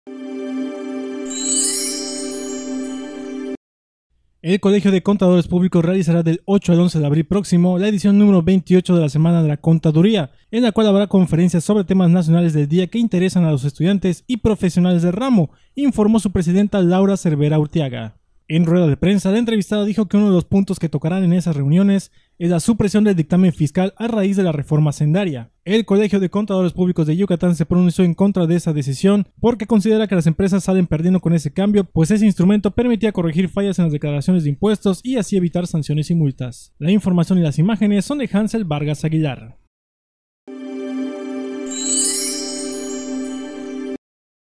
Presentan la Semana de la Contaduría
El Colegio de Contadores Públicos realizará del 8 al 11 de abril próximo la edición número 28 de la Semana de la Contaduría, en la cual habrá conferencias sobre temas nacionales del día que interesan a los estudiantes y profesionales del ramo, informó su presidenta Laura Cervera Urtiaga.